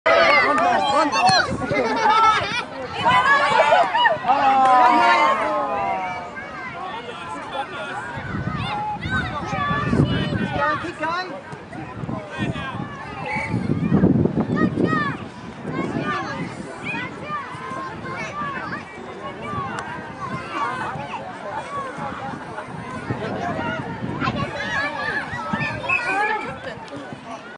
Metro FC Galaxy v W.S. Firebolts - Sportsmanship
I have encoded and edited hours of video, watched some good play, seen some excellent goals.

This incident is the one that sticks in my mind.

Home game Metro Galaxy v W.S. Firebolts